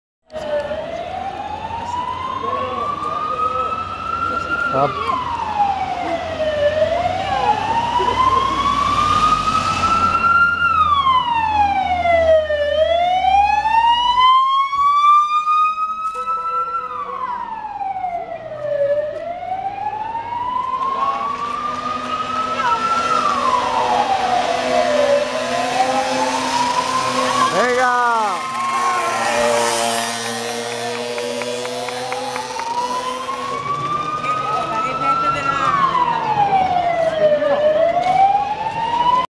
Paso vencedor BTT Hellín.PRODEPOR
Paso a quinientos metros de la línea de meta del vencedor de la prueba, Ochoa Rubio.